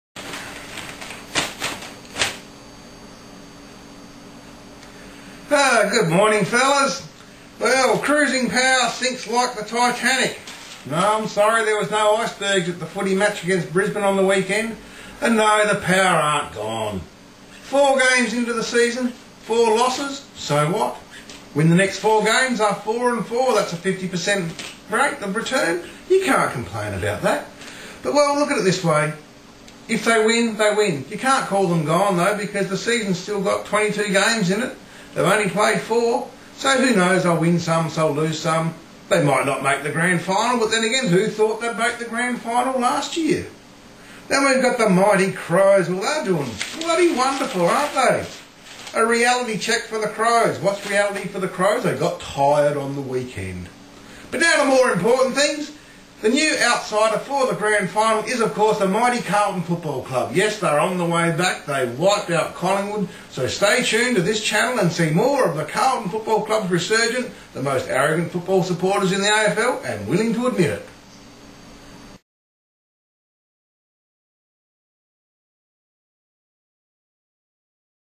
POWER OFF IN 2008 AFL Round 4
A Readers Comments video reply to TIM HILFERTY: It's game over for Port Article on the Adelaide Now site